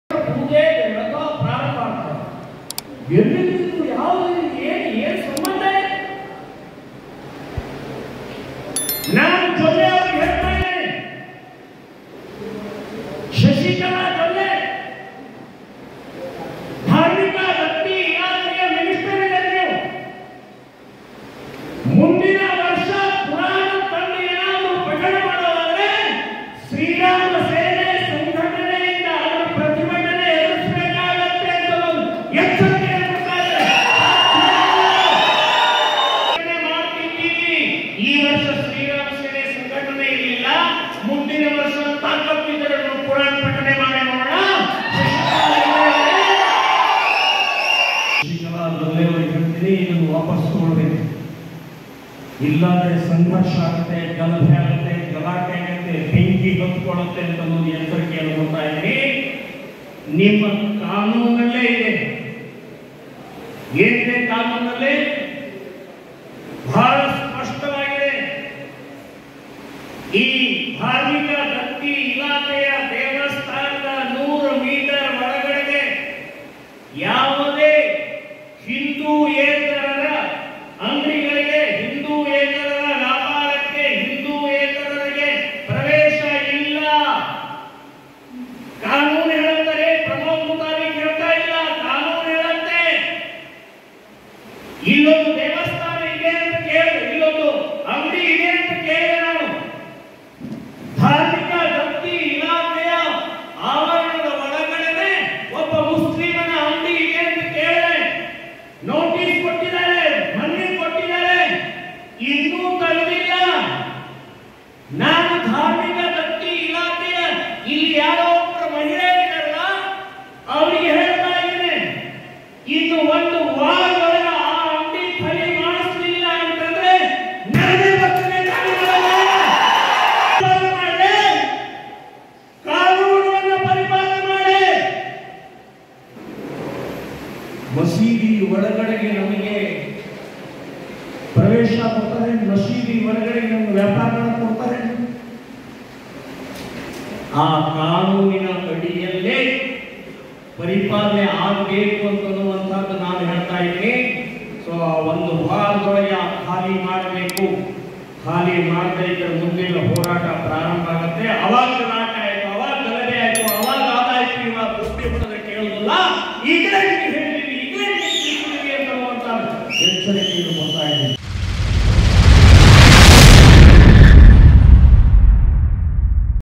ಇನ್ನೊಂದು ವಾರದೊಳಗೆ ಈ ಅಂಗಡಿ ಖಾಲಿ ಮಾಡಿಸದೇ ಇದ್ದರೆ...!
0:00 - Pramod mutalik
0:30 - caution
1:20 - Muslim shops in temples